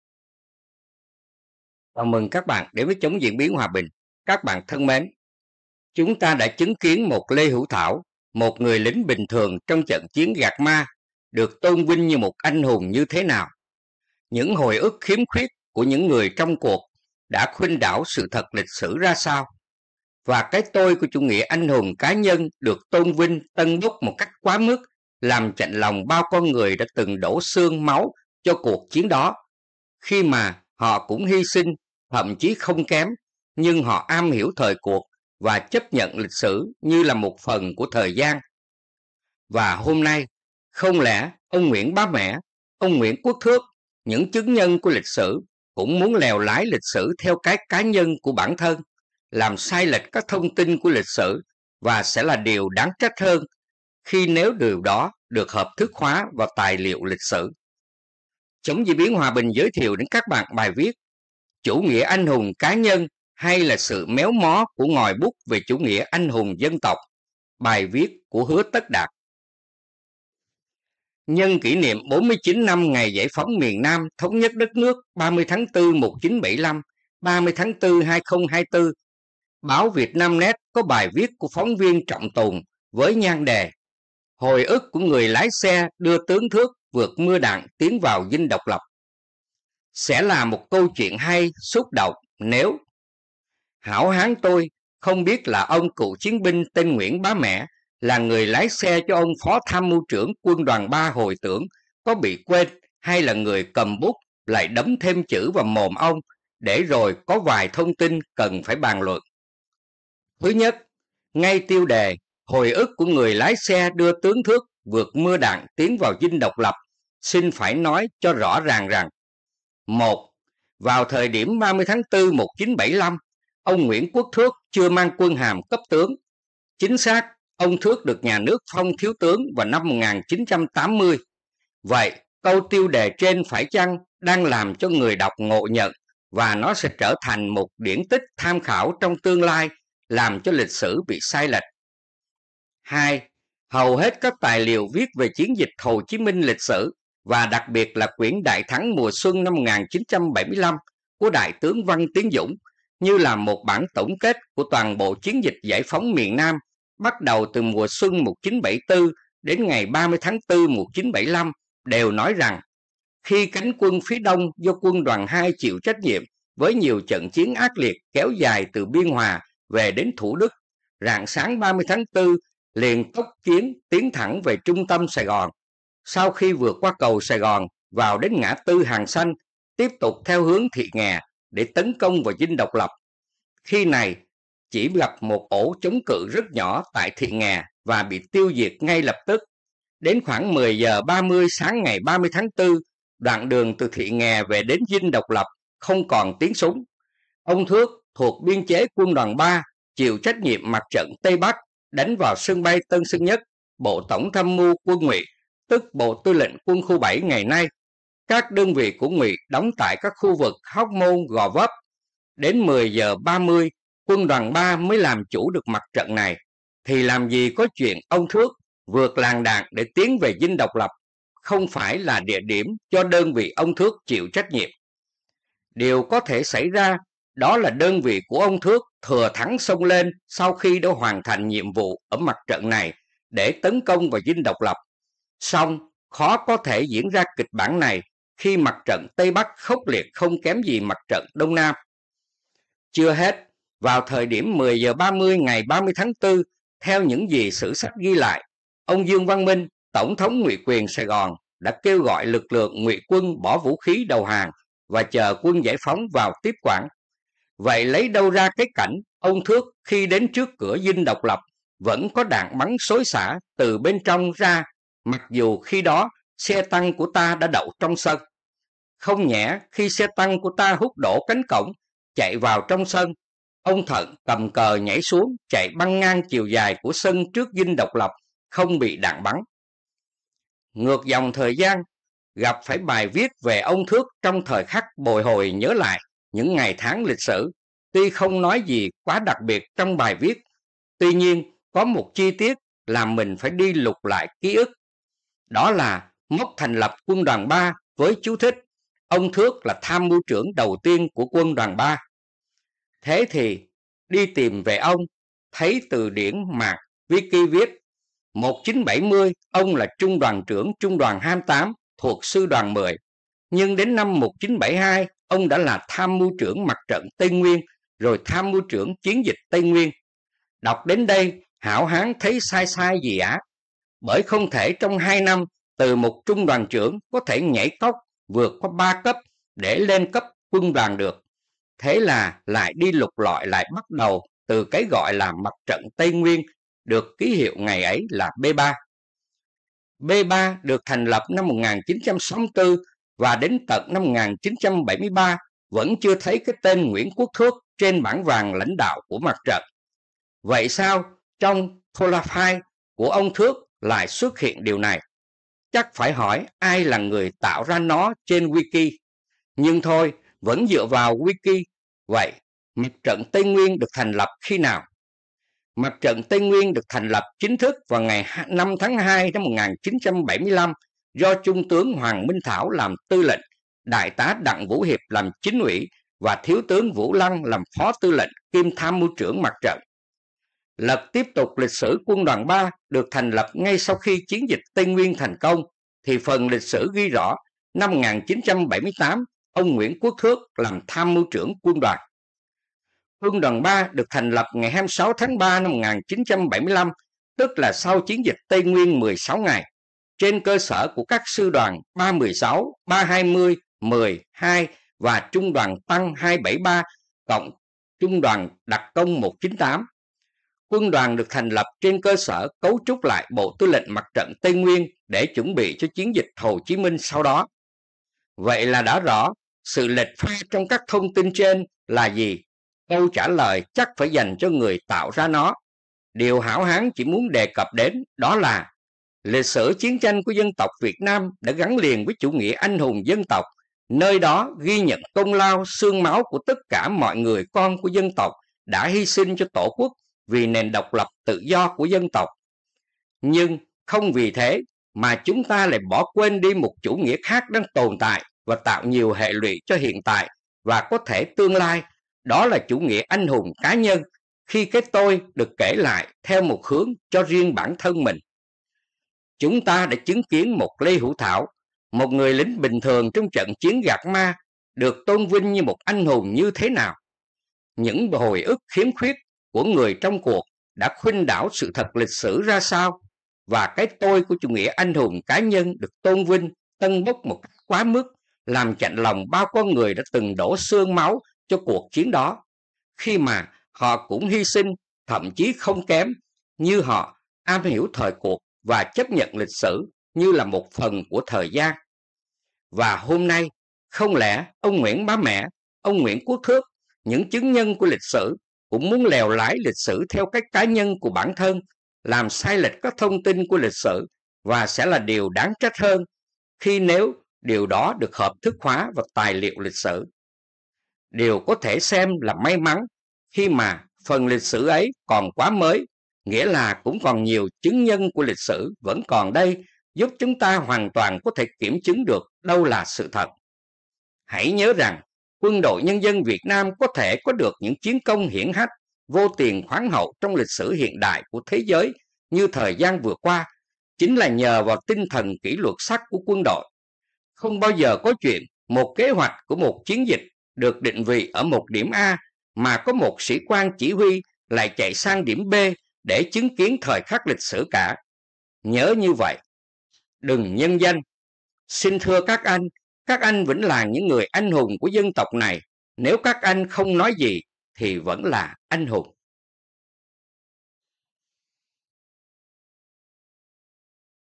CDBHB3637. Chủ nghĩa anh hùng cá nhân hay là sự méo mó của ngòi bút về chủ nghĩa anh hùng dân tộc?
Chủ nghĩa anh hùng cá nhân hay là sự méo mó của ngòi bút về chủ nghĩa anh hùng dân tộc?
Hứa Tất Đạt